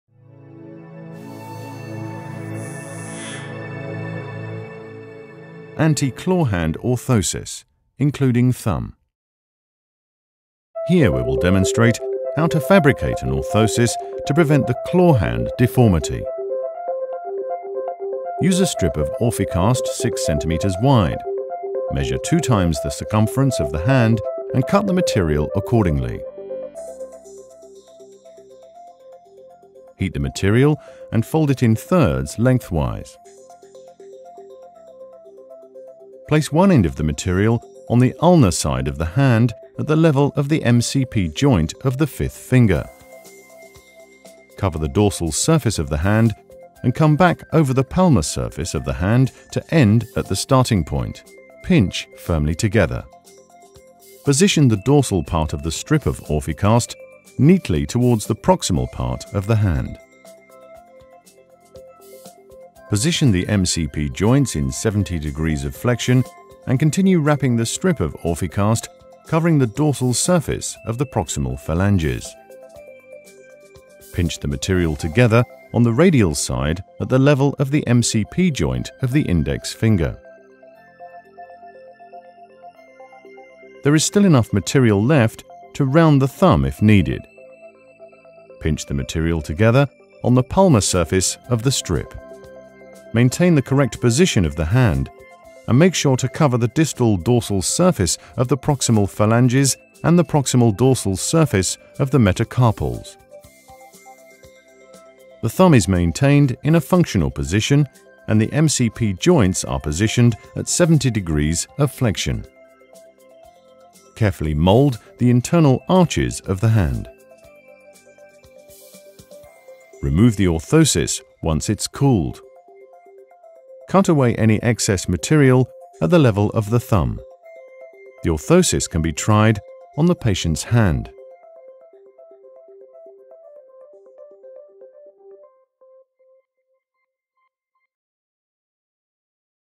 Anti-claw hand orthosis (including thumb) - Orficast Instructional Movie 4
This movie demonstrates how to fabricate an orthosis to prevent the claw-hand deformity.